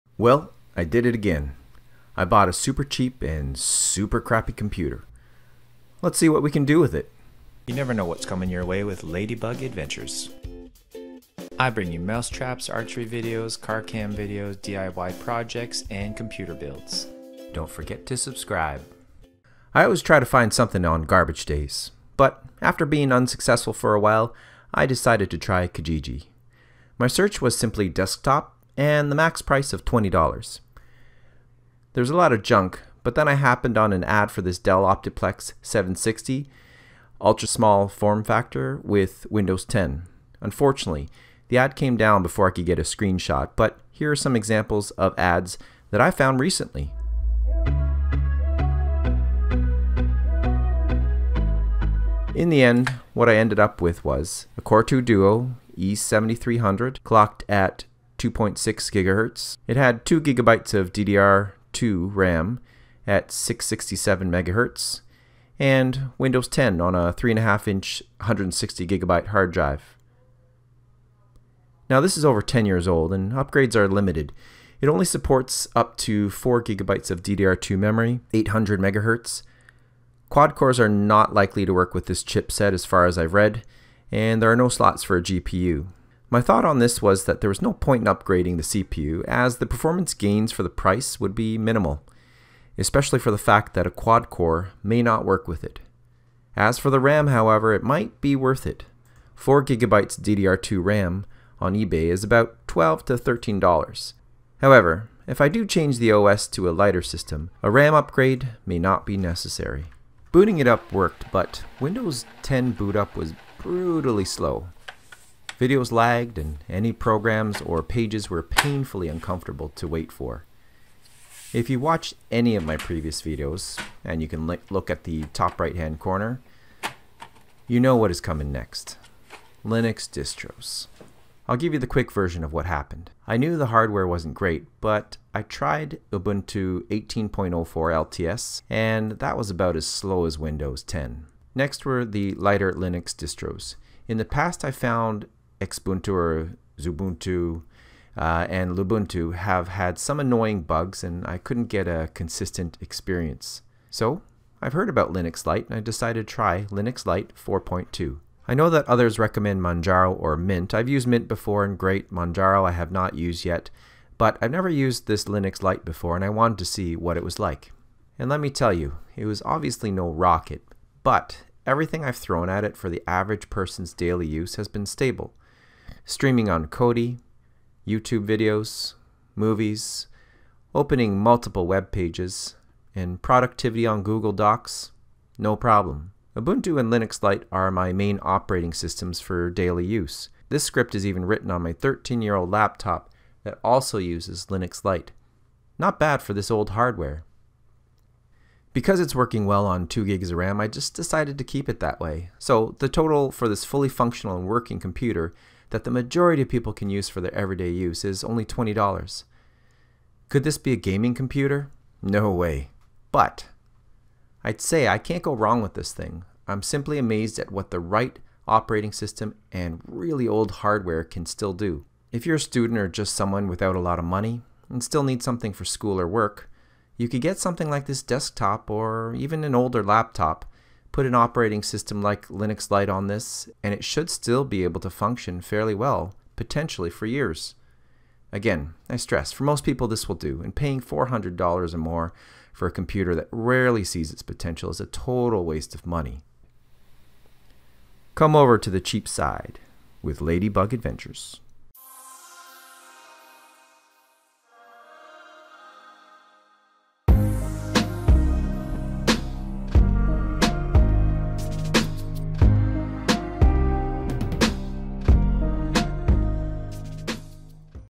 Linux Lite lets this $20 computer live in 2019
I dare you to try this. Get a very old and low spec computer and try Windows 10. Then try linux lite. What was destined for the junk yard is now something that you could actually live with as a daily computer.

If you're a student or someone who doesn't have a lot of cash this can allow you to breathe new life in an old computer.

Please try this before you throw away a perfectly good computer, or buy an old computer and actually give a light Linux distro a try.